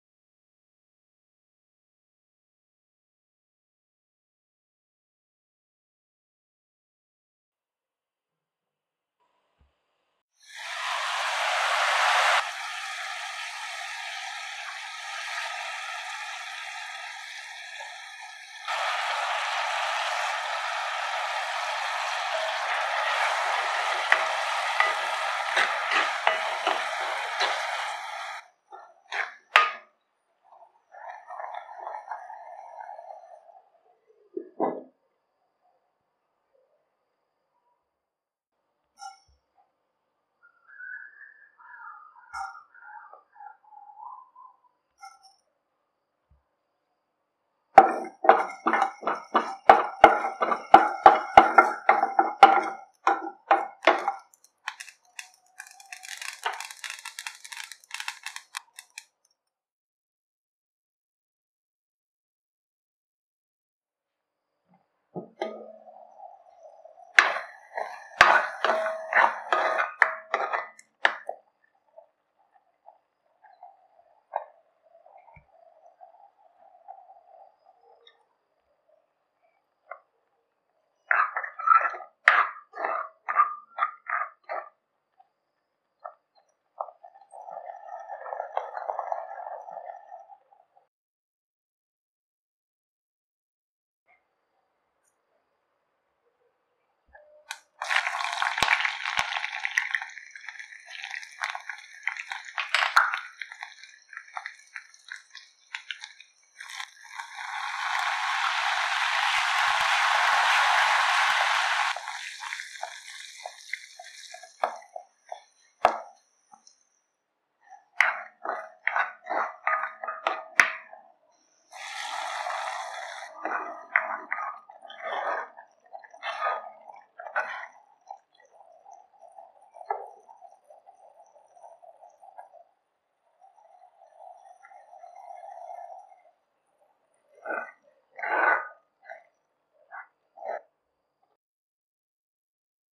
Tomato Thokku / Simple & Tasty Thakkali Thokku
Tomato Thokku

Ingredients
Turmeric powder
Salt
Fenugreek – ½ teaspoon
Cumin seeds – ½ teaspoon
Tamarind juice of amla size
Gingelly oil – 3 tbl spoon
Fenugreek
Curry leaves